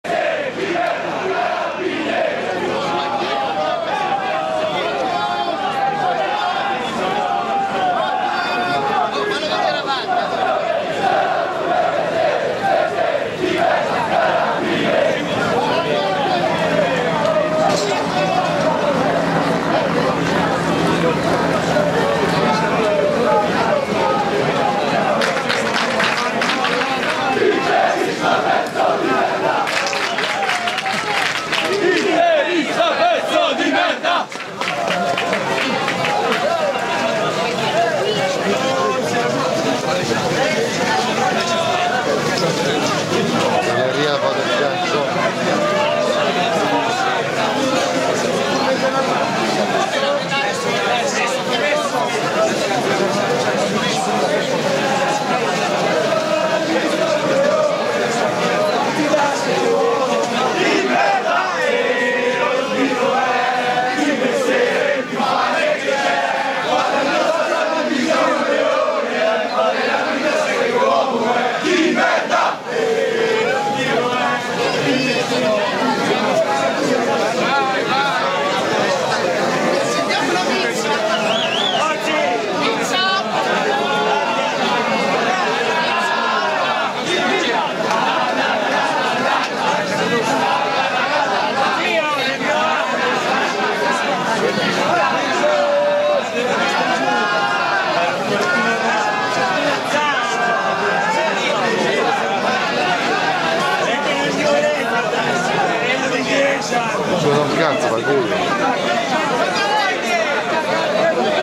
Cesena Milan 1-3 Curva Sud Milano ''CORI NEL TUNNEL BUIO CHE CONDUCE AI PARCHEGGI'' IN FULL HD''.
Cesena 19 Febbraio 2012 Cesena Milan 1-3 momenti in Curva Sud Milano prima e durante la partita.